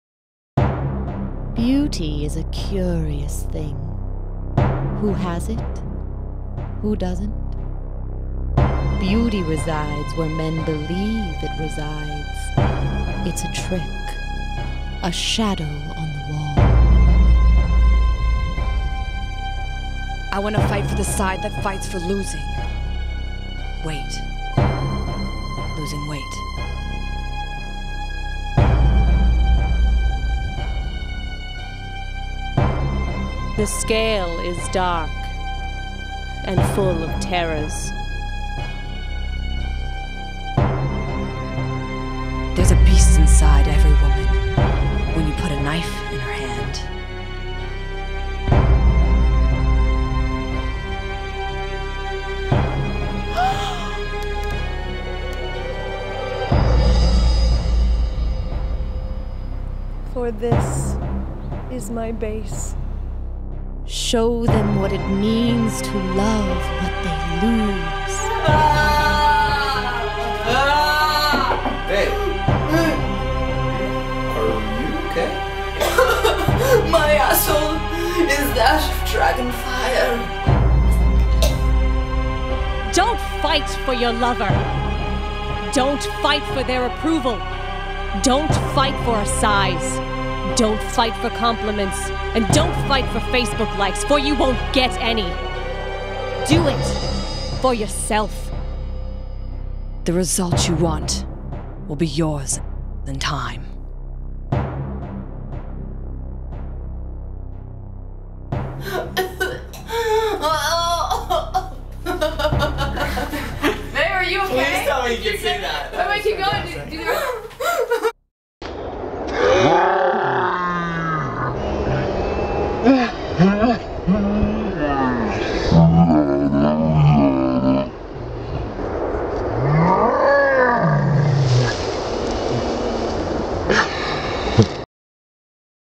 Game of Thrones Official Trailer - Summer is Coming (Parody)
Look, we love Game of Thrones, but we live in Los Angeles and lets face it, winter is never coming...however summer, summer is coming and it's serious business.  Lots of hardcore preparation.  We thought we'd have some fun and parody the official trailer.

Written and Directed by Lydia Dorsey
Shot and Edited by Alex Reeves (Point of Blue Prod)
Featuring Amber Sweet and Nikki Mckenzie of Full Frontal Females

Amber Sweet
Kristen Quintrall
Lydia Dorsey
Nicole Wyland
Nikki McKenzie